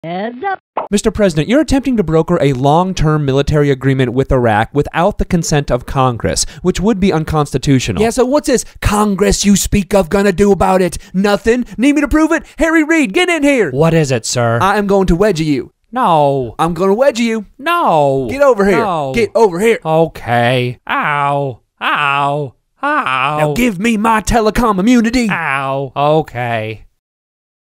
Headzup: George Bush, Harry Reid, And Unratified Treaties
President Bush is asked about setting up a long term military arrangement (or treaty) with Iraq without the consent of Congress.

Download daily Catch and Release Comedy&trade; political cartoons to your mobile phone or iPod